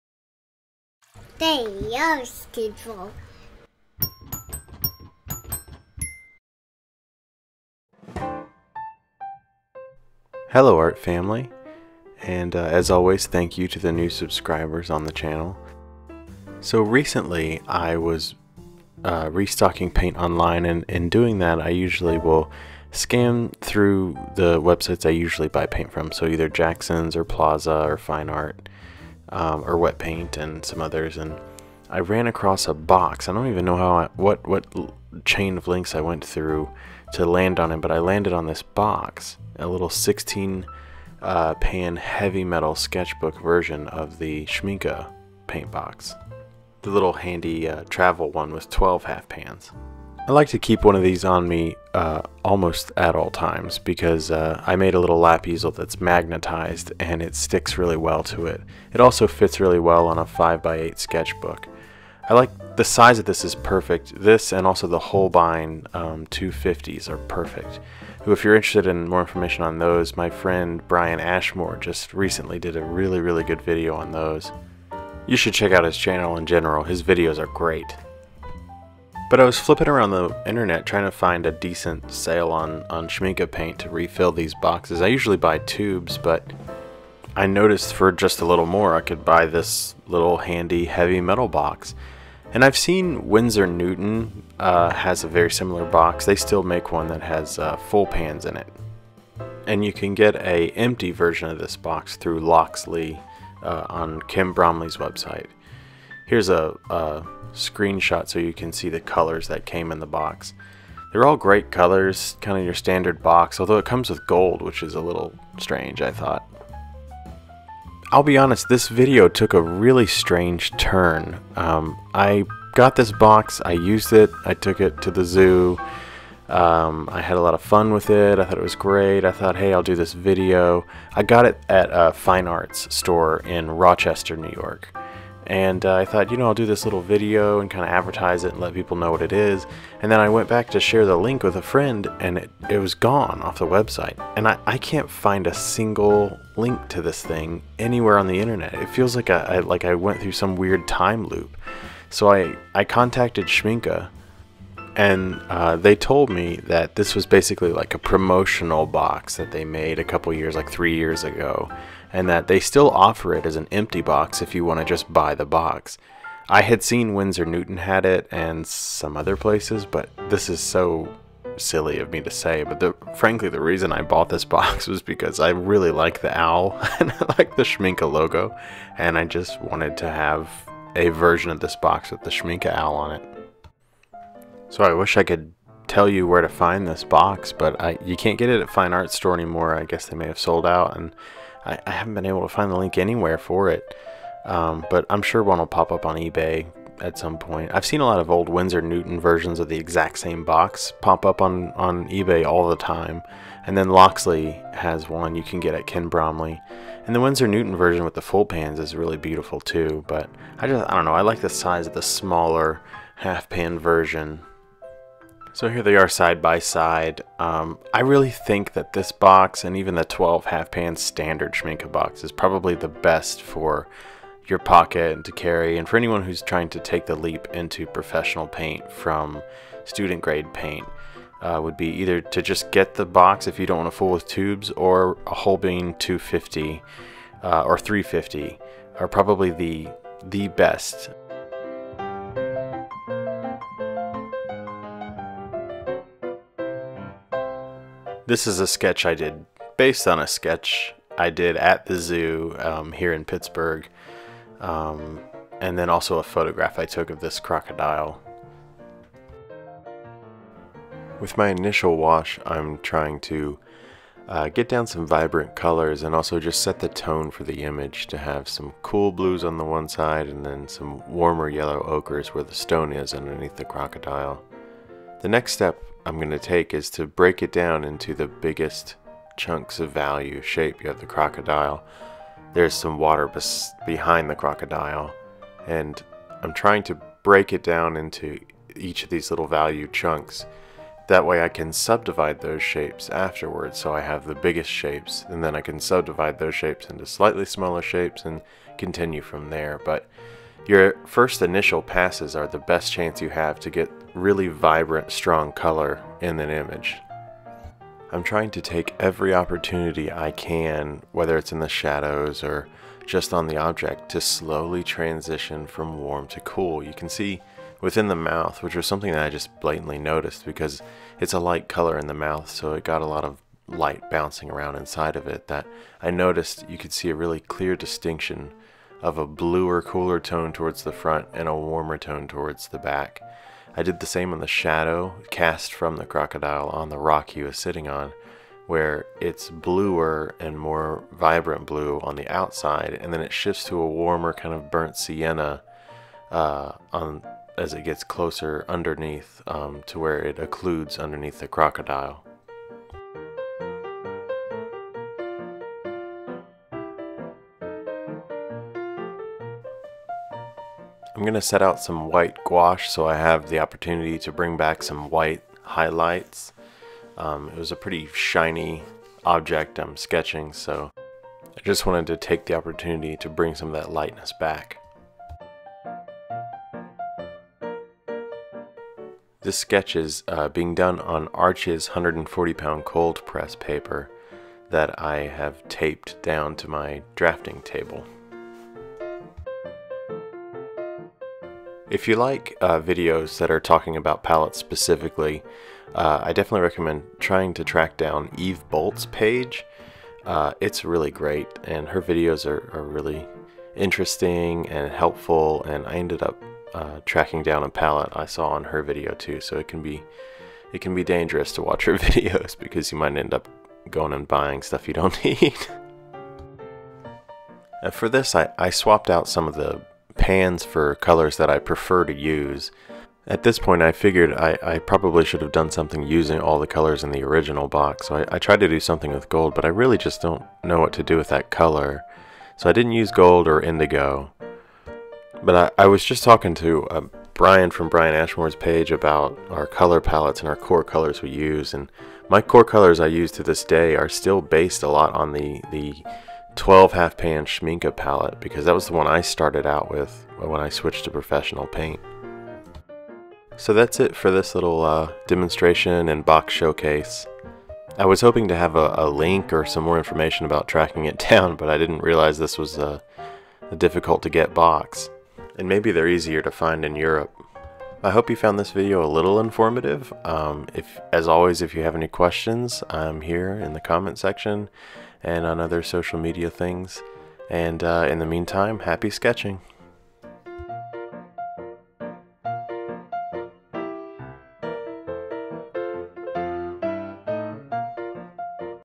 Schmincke Horadam 16 Half-pan Heavy-weight Sketchers Box and Sketching Demonstration
Hey Art-family,
I was refilling paint online and as usually flipping through the various sites I buy from to find the best sale. I came across this box at Fine Art Store's website and bought it. I was hoping to share a link on here but I can't seem to find any link to it again. I did find a link through Amazon but I would contact the seller first to make sure they're not sold out:
I reached out to Schmincke to ask about the box and they told me it was a promotional box they did 2-3 years ago but that they still offer it as an empty box.

Hope you found the video helpful!
Happy sketching,
Jared